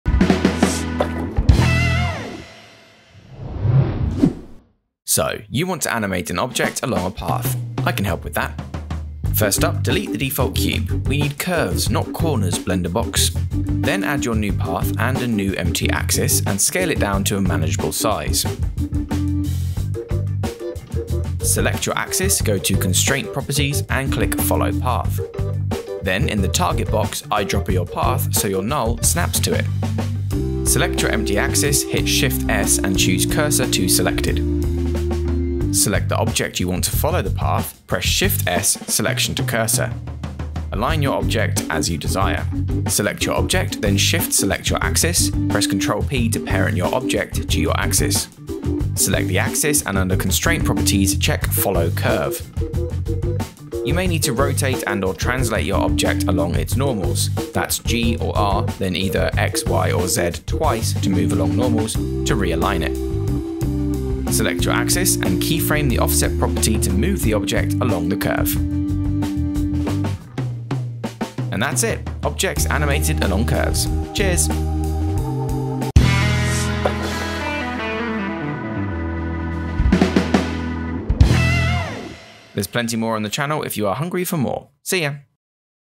Animate Objects Along Paths/Curves in Blender
In this video we take a look at the simplest way to animate objects along paths or curves in Blender! Let's jump right in!